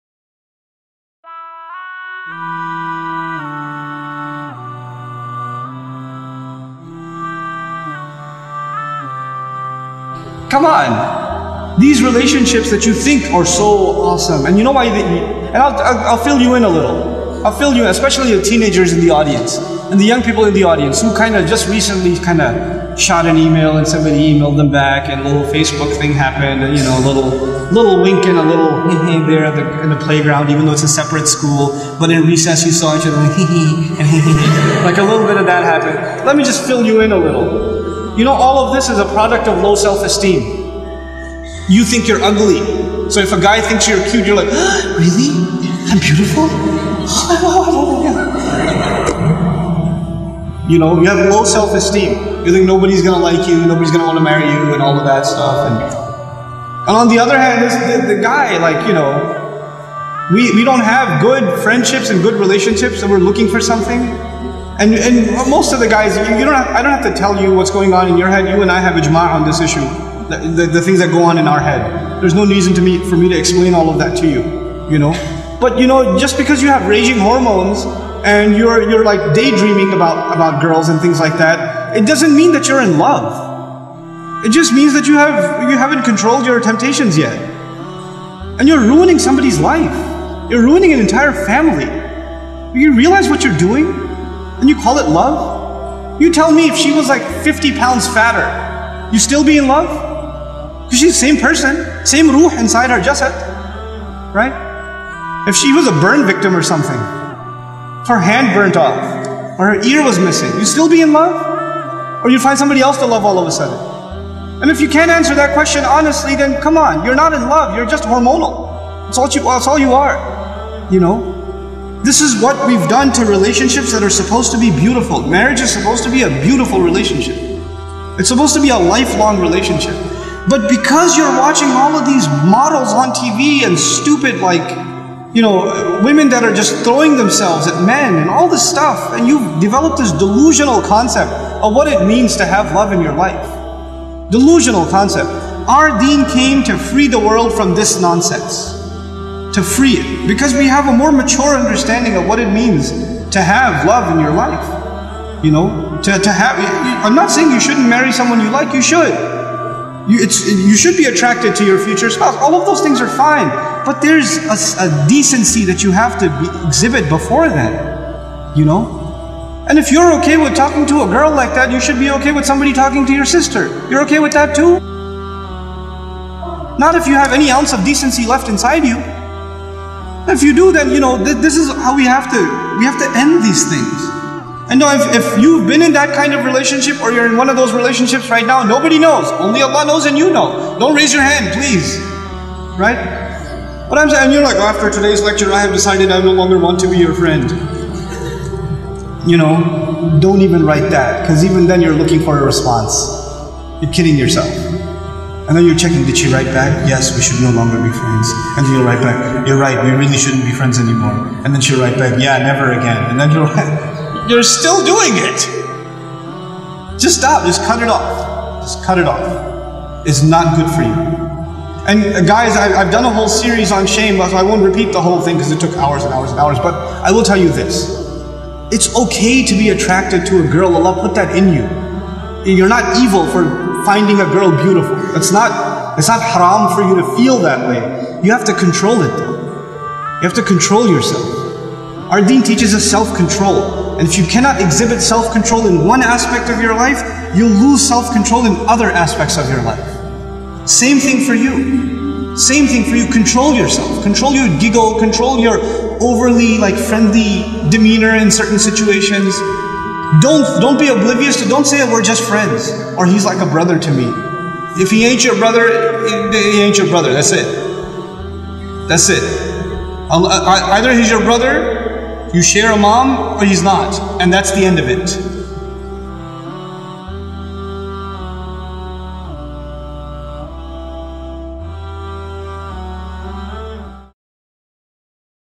You're Not in Love - You're Just Hormonal - Islamic Reminder
Personal Facebook Brother Nouman Ali Khan discusses the delusional concept of "Love" that we have in today's society.